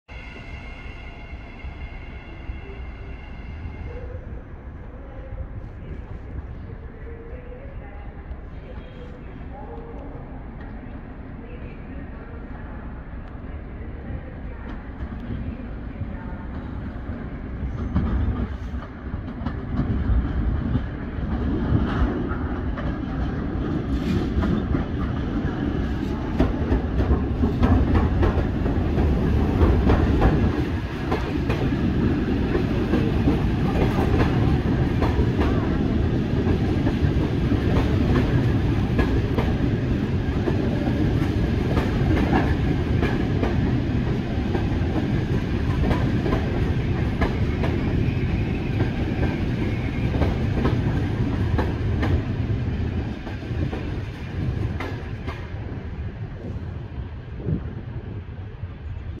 LTG Link Siemens Er20cf- 023
Lithuanian railways (LTG-Link) passenger train Siemens Er20cf- 023 with classic wagons (Klaipėda- Vilnius) arrive to Vilnius station. Lithuania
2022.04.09